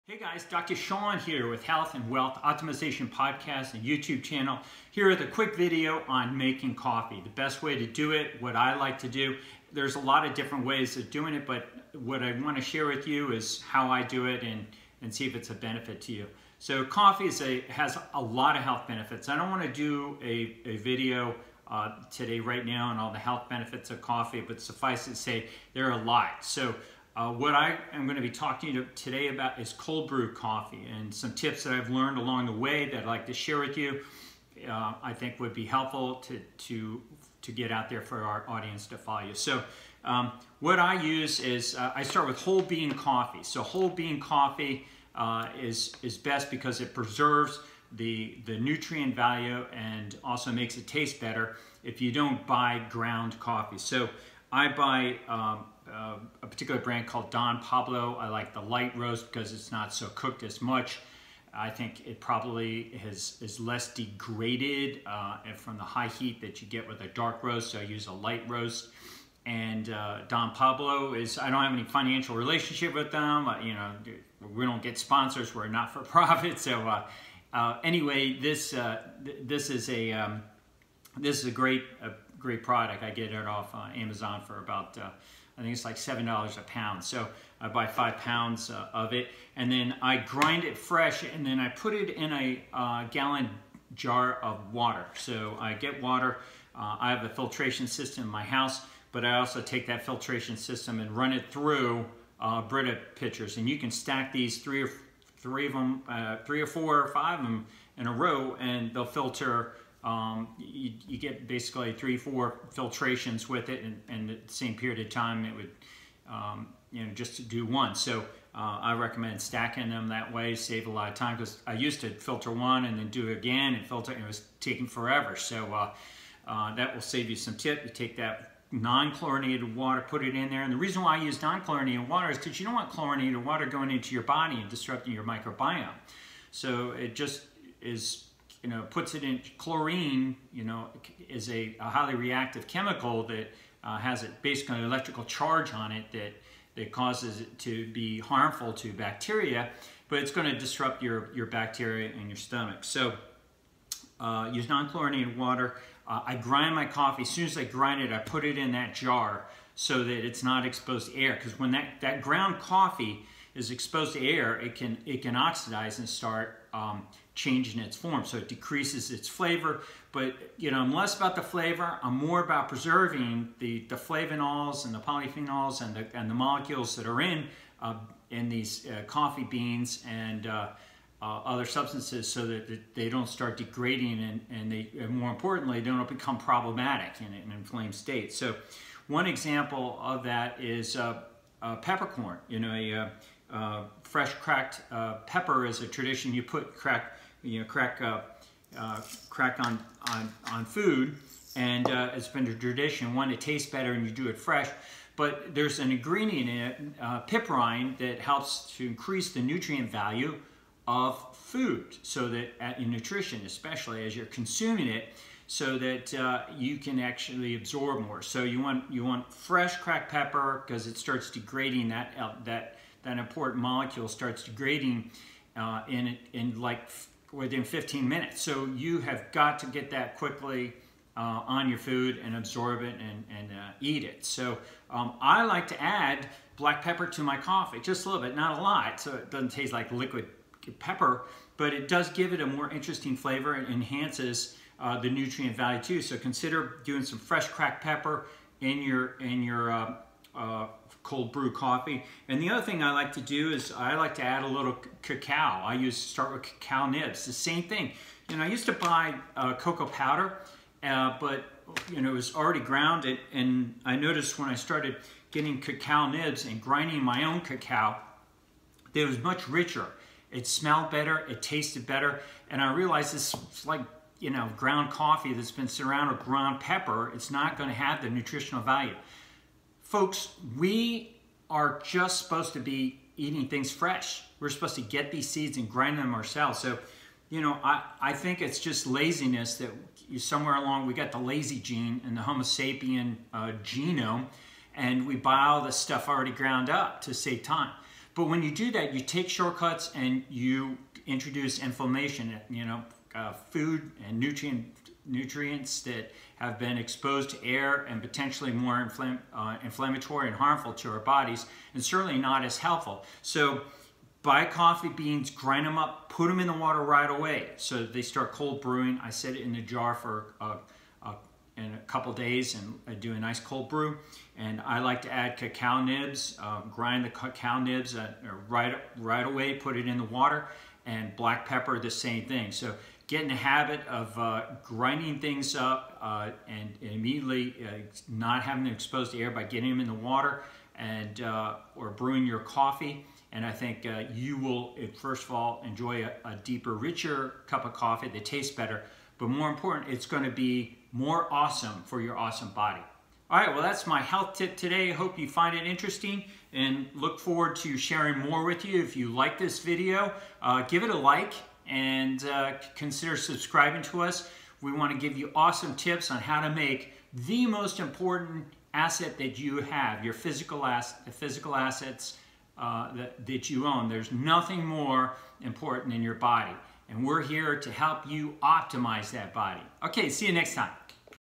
How to prepare Coffee, Cacao and Black Pepper so it is Healthier for you
This short video explains how to prepare coffee, cacao and black pepper so it is healthier for you and possibly even limit its potential for harm. Coffee beans, Cacao beans and even black pepper perfectly preserve the amazing health optimizing elements inside them until it is processed which then can expose you to potential inflammation. Learn how to take steps to limit this.